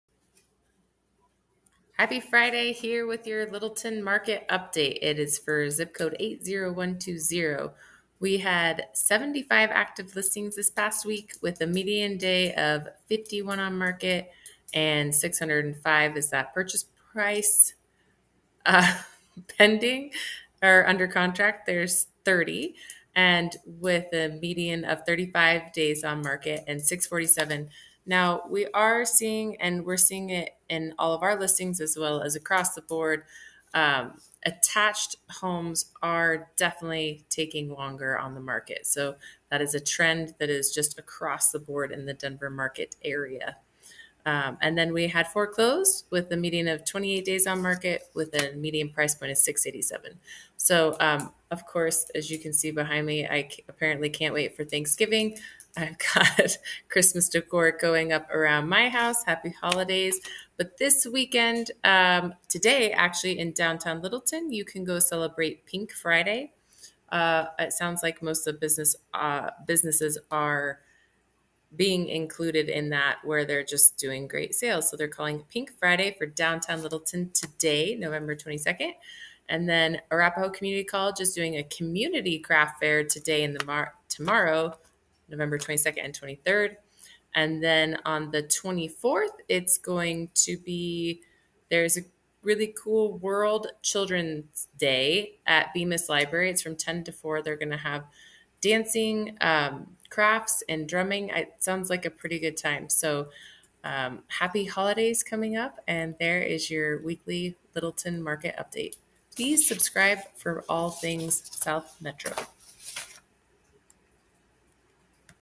Your Littleton, CO Market Update + local events!
Happy Friday! I am here with your Littleton Market for the past 7 days!
‼️ Active Listings: 75
Median Purchase Price: $605,000
Median Days on Market (DOM): 51
Under Contract: 30
Closed Homes: 4
Median Close Price: $687,000

🩷 TODAY (11/22) is Pink Friday in in Downtown Littleton! Most businesses are participating with some wonderful sales!

🎨 Arapahoe Community College is doing a Community Craft Fair today & tomorrow, 11/22 & 11/23

📚 Bemis Library is celebrating World Children's Day with dance, music, crafts & more! Saturday, 11/23 from 10:00 AM - 4:00 PM